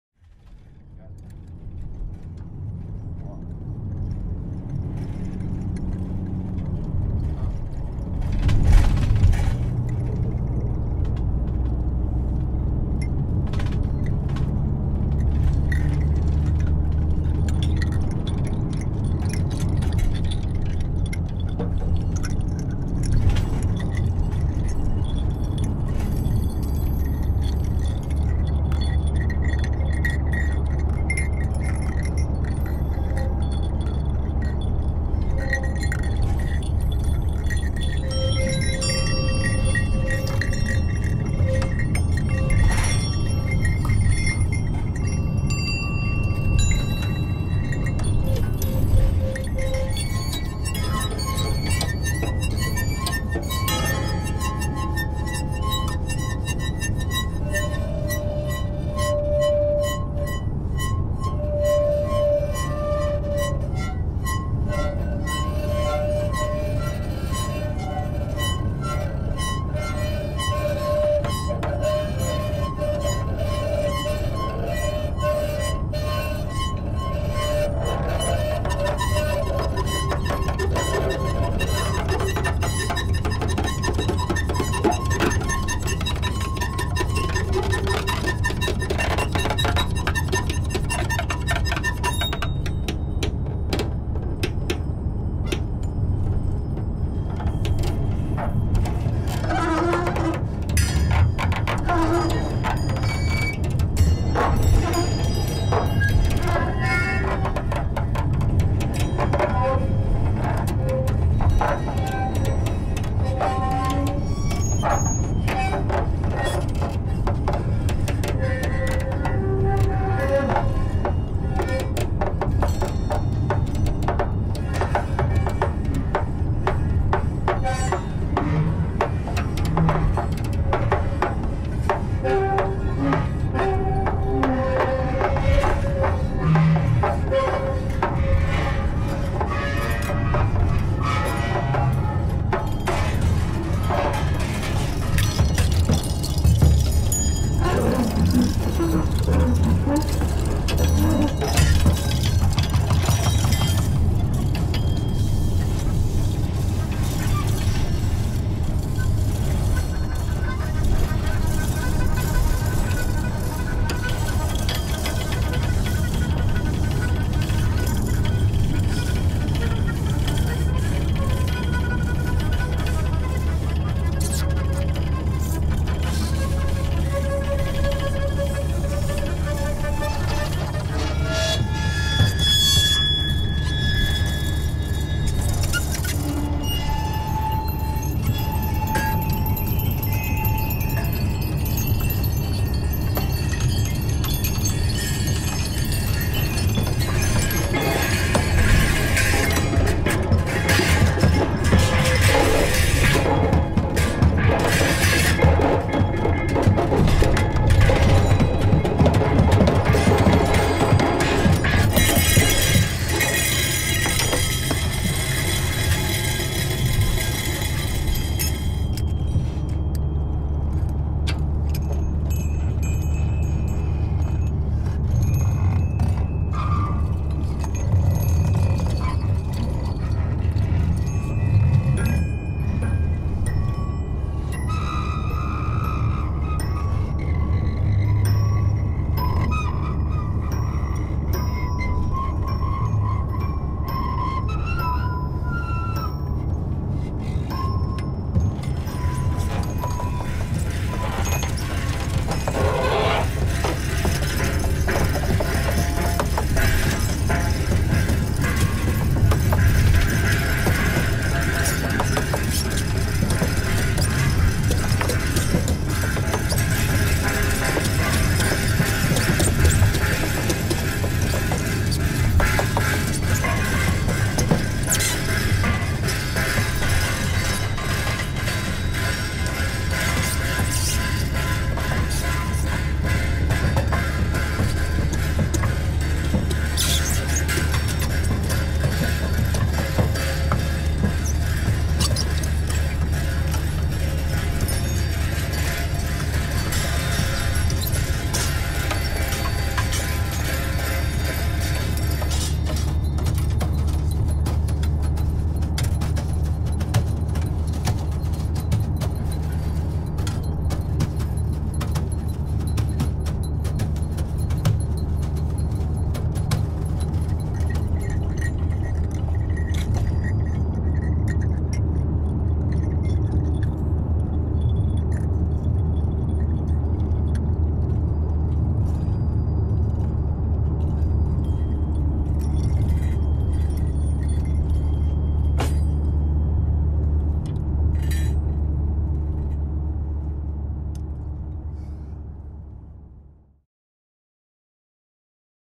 35,04km of music in loop
"35,04 km of music in loop" was shoot on the peripheric boulevard of Paris in march 2011. The duo Diatribes improvises in a car, filmed by Jérôme Fino, during the time it takes to browse the entire loop of 35,04 km.
The original video is 25 min long, here is an extract.

"35,04 km de musique en boucle" a été tourné sur le boulevard périphérique de Paris en mars 2011. Filmé par Jérôme Fino, le duo Diatribes improvise dans une voiture durant le temps qu'il faut pour faire la totalité de la boucle de 35,04 km. La vidéo originale est 25 min, voici un extrait.